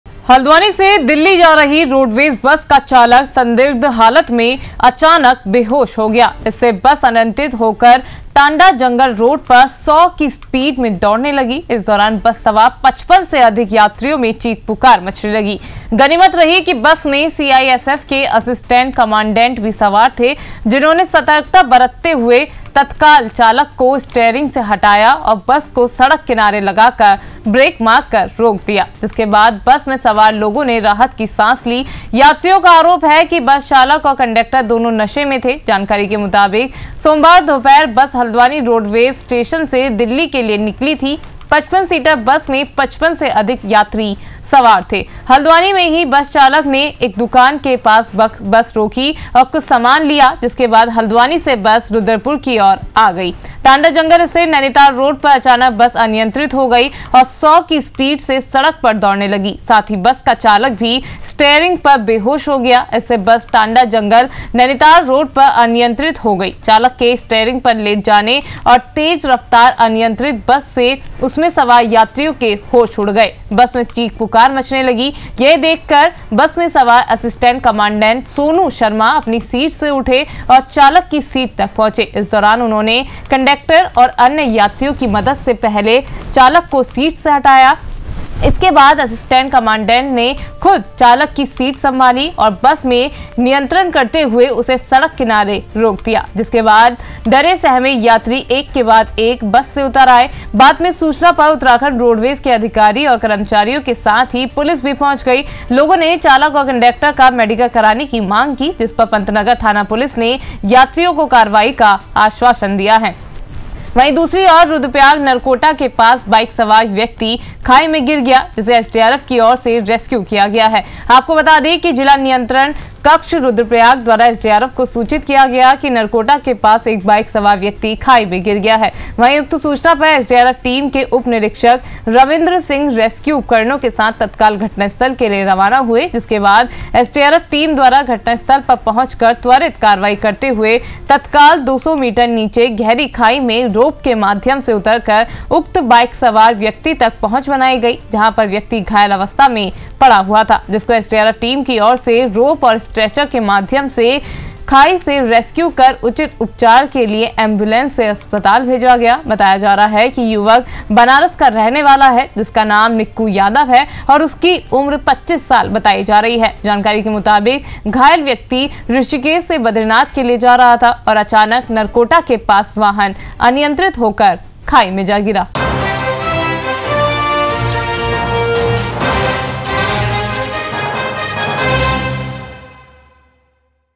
100 की स्पीड में हुआ हल्द्वानी बस का चालक बेहोश, अचानक अनियात्रित होकर खाई में गिरा वाहन|| JJN News
JJN News Nainital,
JJN News,